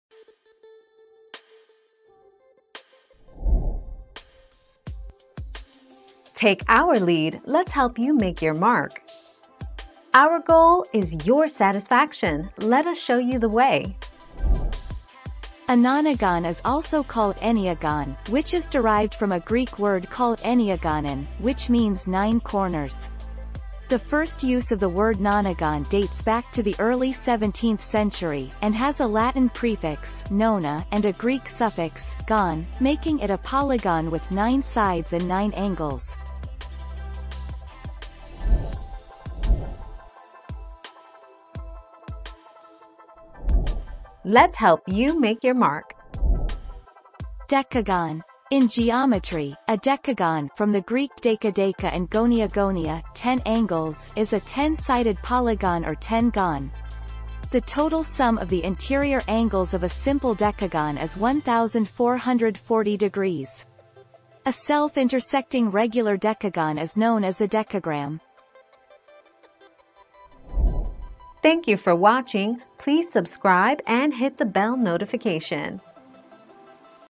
Is there a shape with 9 sides?
00:00 - Is there a shape with 9 sides?
00:40 - What is a 10 sided shape?

Laura S. Harris (2021, March 8.) Is there a shape with 9 sides?
    AskAbout.video/articles/Is-there-a-shape-with-9-sides-228517